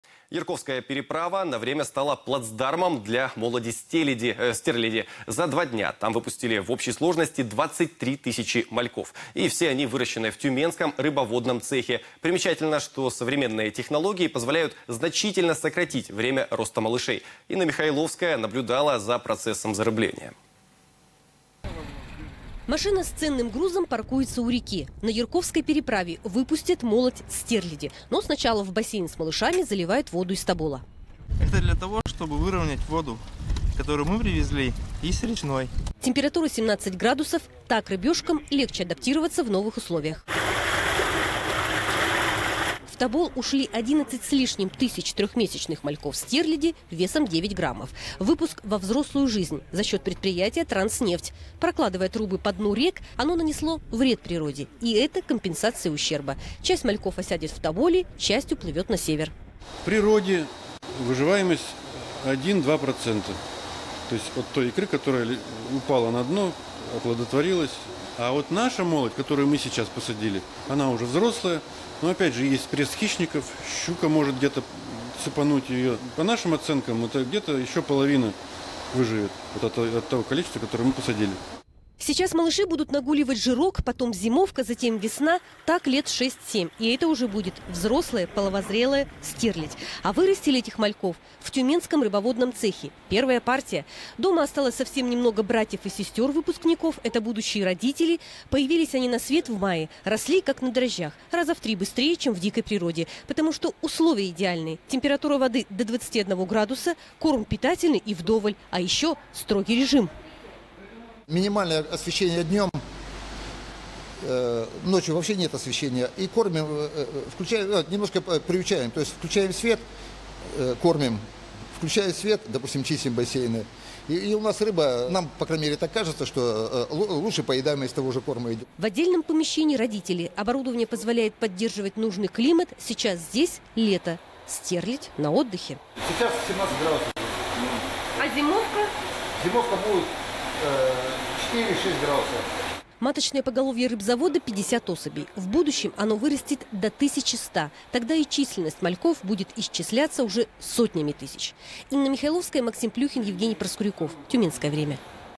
Будем со стерлядью
Ярковская переправа на время стала плацдармом для молоди стерляди. За два дня там выпустили в общей сложности 23 тысячи мальков. И все они выращены в тюменском рыбоводном цехе. Примечательно, что современные технологии позволяют значительно сократить время роста малышей.